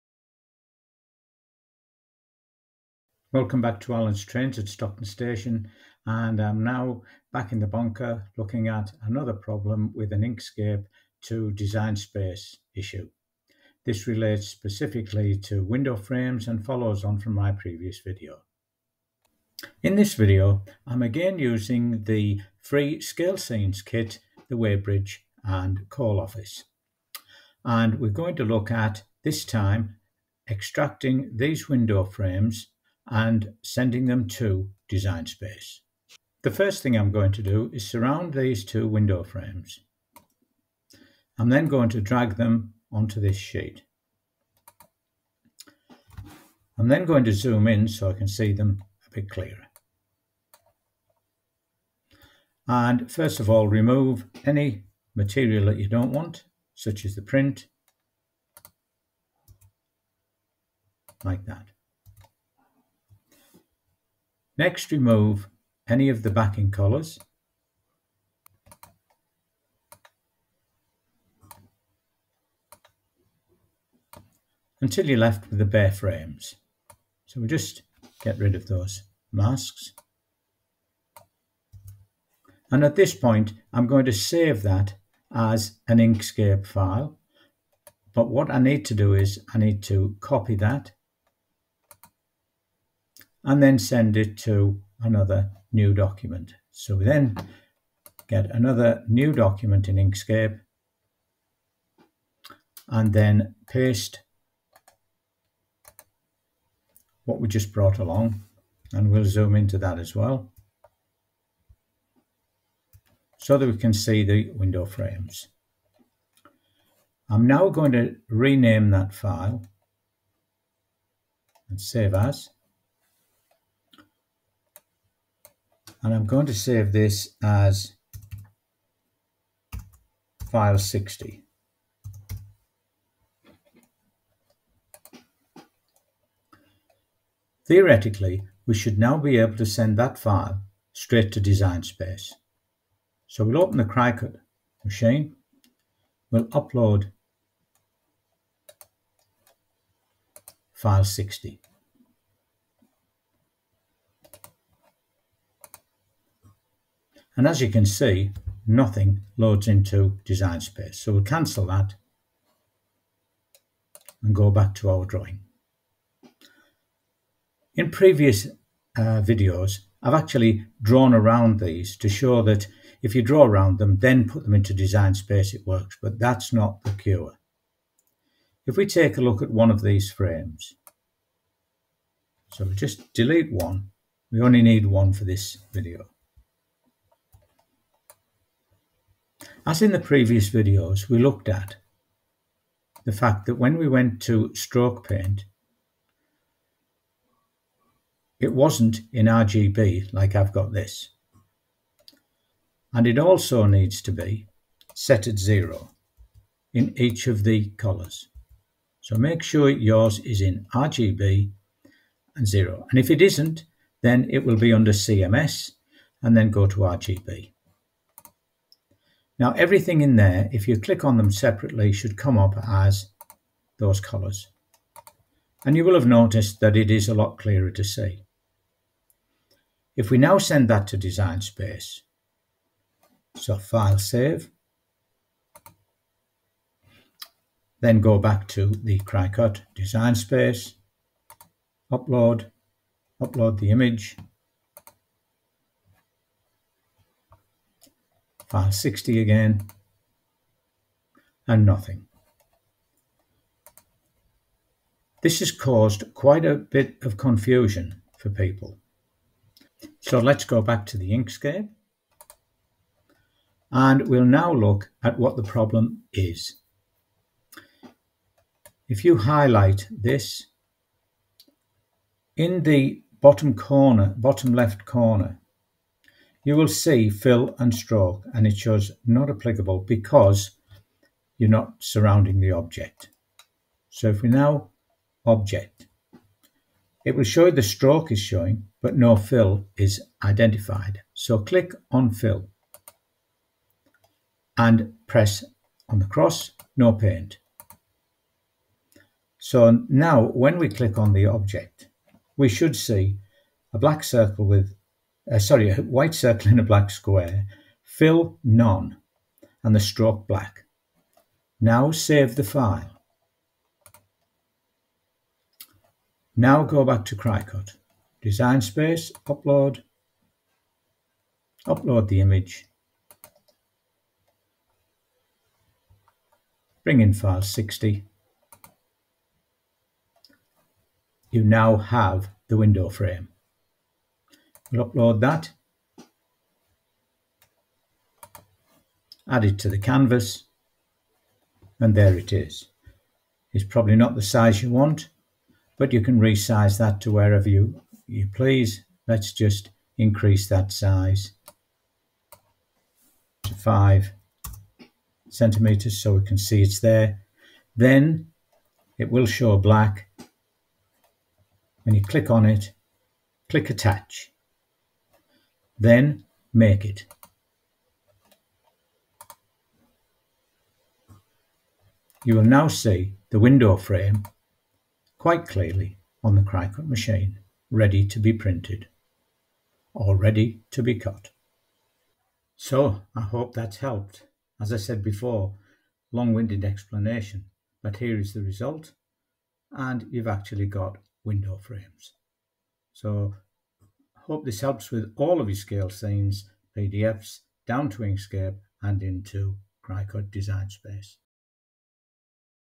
Inkscape PDF to CRICUT Design Space Issues 2
This video is part 2 dealing with downloaded PDFs from Scalescenes are edited and will not load to a Cricut Cutting Machine (DesignSpace).
This is NOT a Scalescenes issue and is based on settings within Inkscape relating to STROKE, FILL and colour settings being incompatible with Design Space.
The video shows the problem, its causes and the solution.